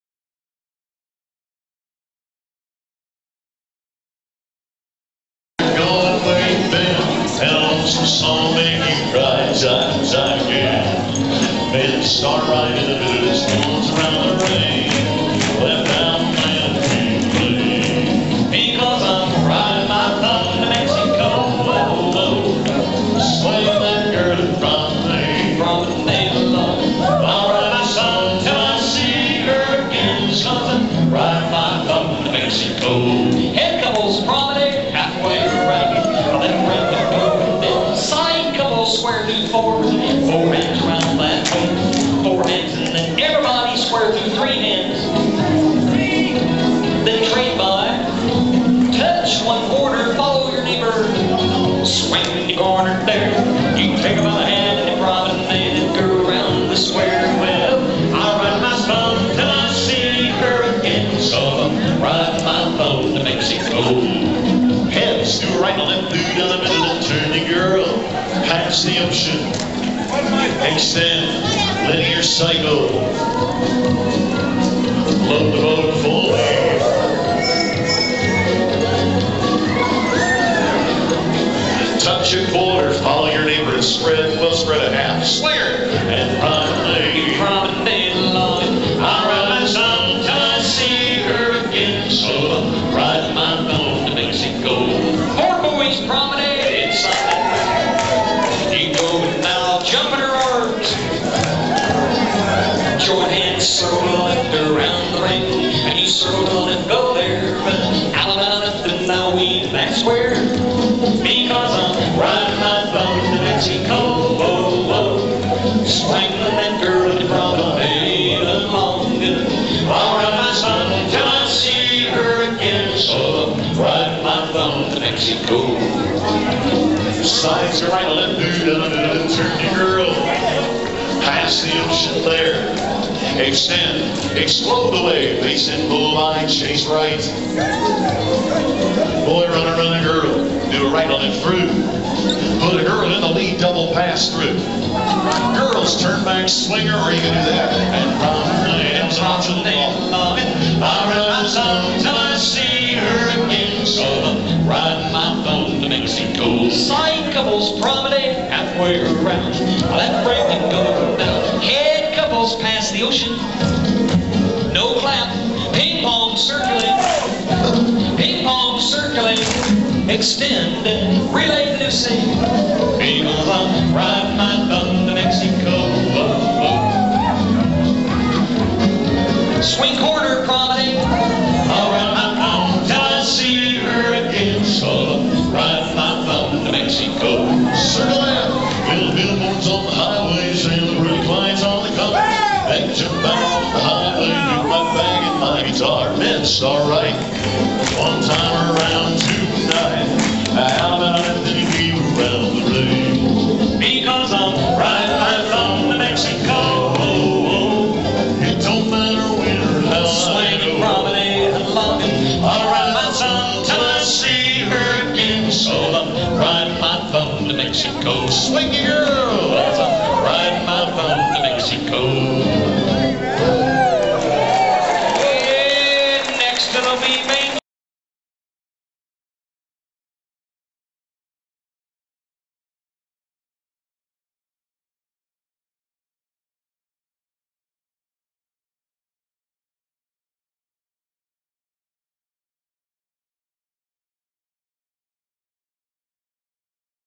Square Dancing Modern Western Style at May Madness 2011 in Golden, Colorado
Square Dancing modern western style at May Madness 2011 in Golden, Colorado with Tom Roper and Mike Sikorsky